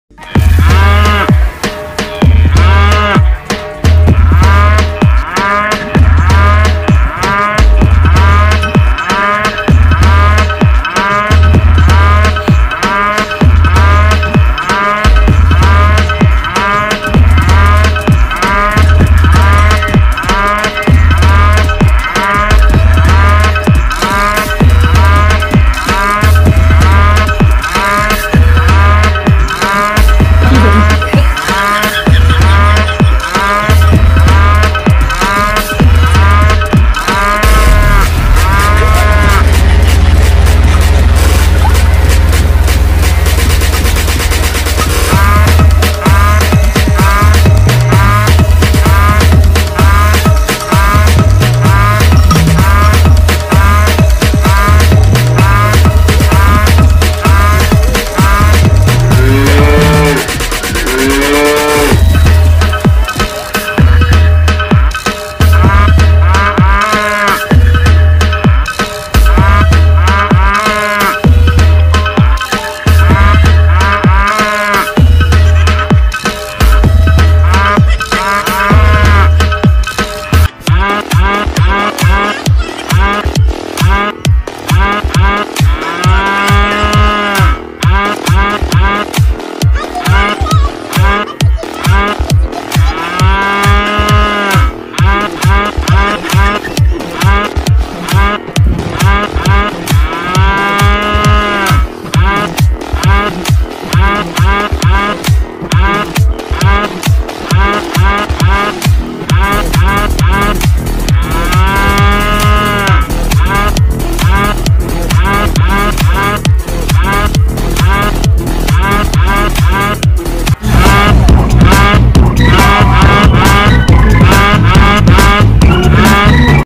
honey cow🐄 dance videos 2022 funniest cow cows dance
hi friends
in this video we will see the cow dance
and sound of cow on dance video
you know that the cow is the best
animal of animal kingdom
best lovely cow
funny cow
cute cow

happy cow
thanks